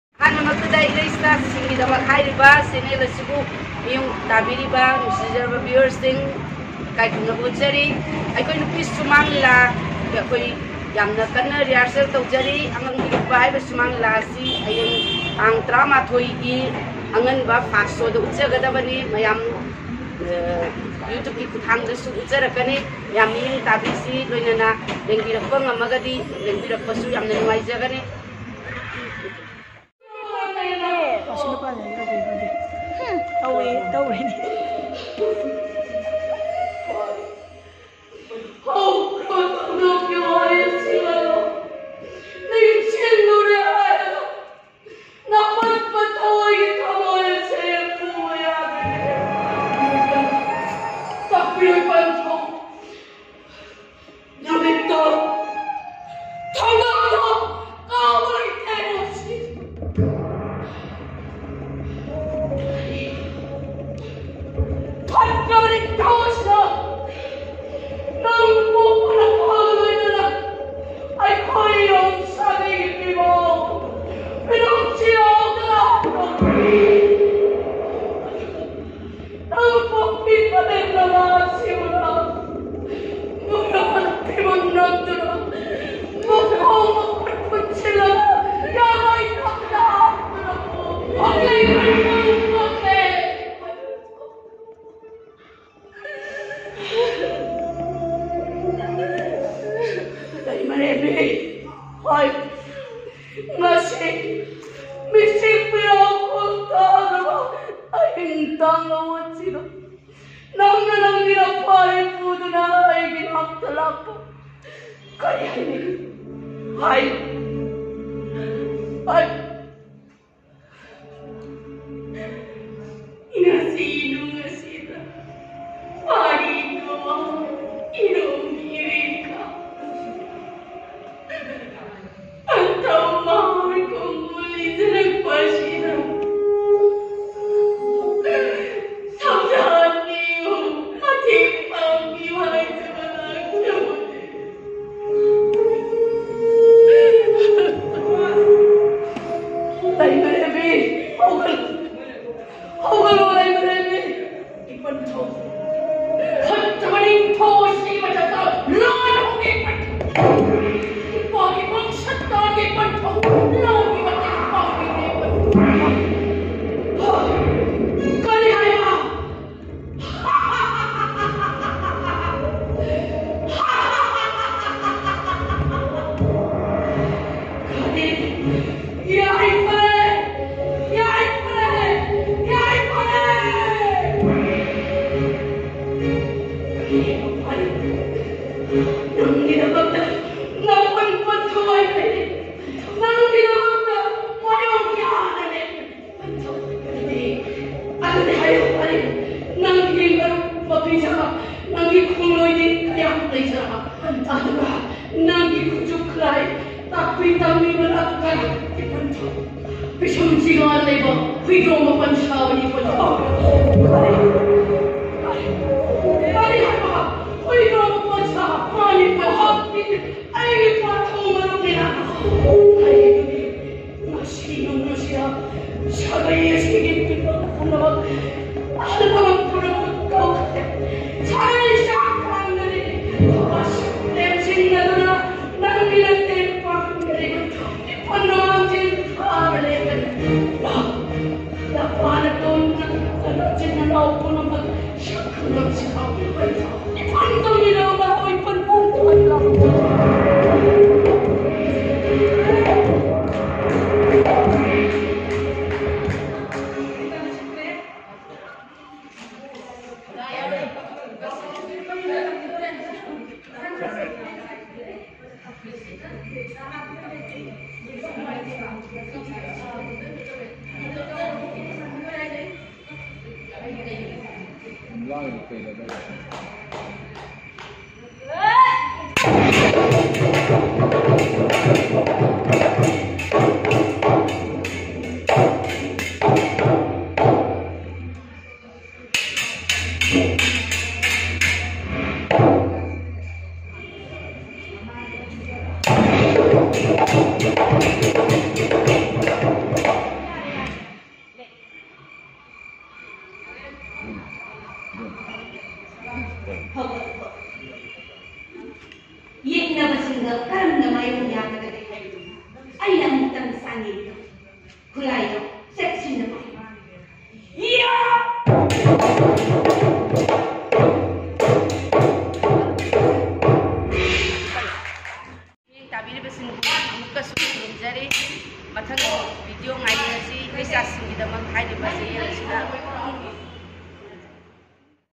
Festival Thoknabagi damakta Full rehearsal pangkhokpa
Mapung phaba shumang lila ama oinaba hotnabada music support toubana henna mapung phahalli maram aduna full rehearsal da music support touba tammi